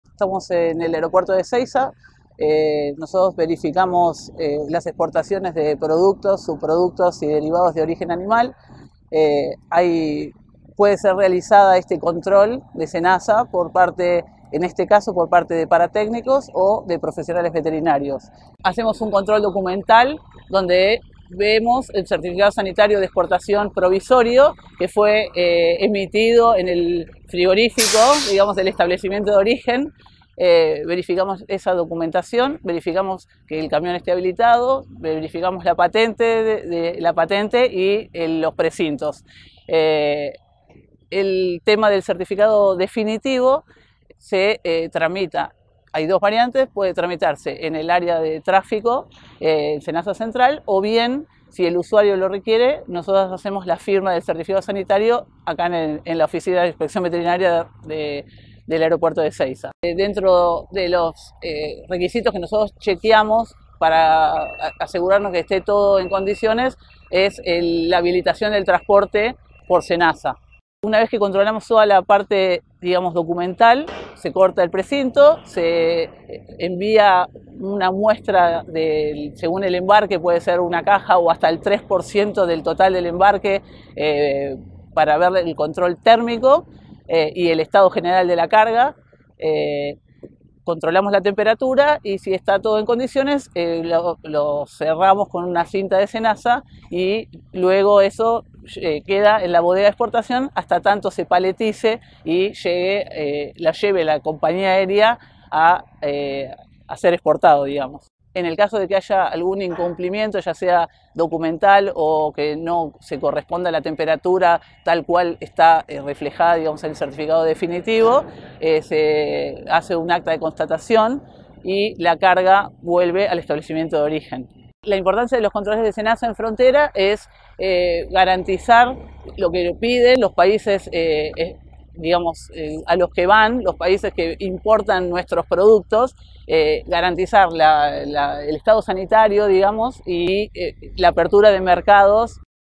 Exportaciones de carne
En el aeropuerto de Ezeiza, el Senasa controla que los envíos de carnes frescas cumplan los requisitos de los a distintos países de destinos.